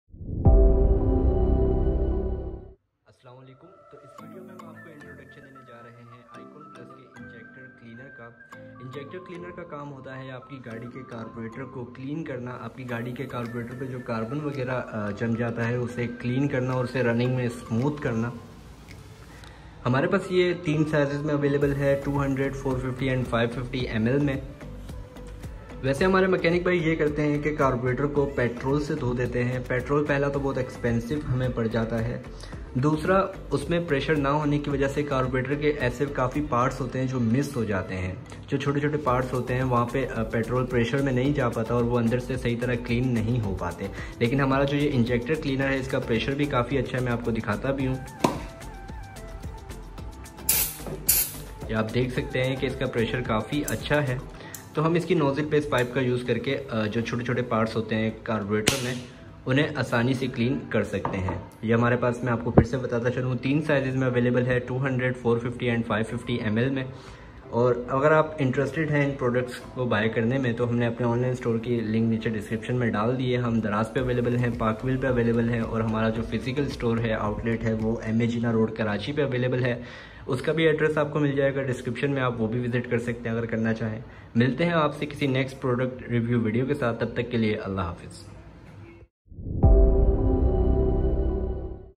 ICON PLUS INJECTOR CLEANER REVIEW
Available in 3 sizes 200ml,400ml,550ml

PRODUCT DETAILS OF ICON PLUS INJECTOR CLEANER - 550ML

Cleans clogged injectors
Cleans the entire fuel delivery system
Improves engine efficiency and performance
Extends engine life

It can clean gunk, varnish, and dirt from choke valves and carb. Removes deposits quite seamlessly to increase engine performance and fuel economy. Reduces high exhaust discharge, hard starting, and stalling.

DIRECTIONS:
Shake well before use.
Carburetor: Remove the air filter, start the engine, spray at a distance of 15-30cm in all parts of the carburetor, then leave the engine with an increased number of revolutions to run for about 2-3 minutes.
Fuel system, Injectors: Spray into fuel tank about 15 seconds before refilling tank.

Outlet Address:
Office No.18, 8th Floor, Rimpa Plaza, M.A Jinnah Road, Preedy Quarters,
Karachi, Sindh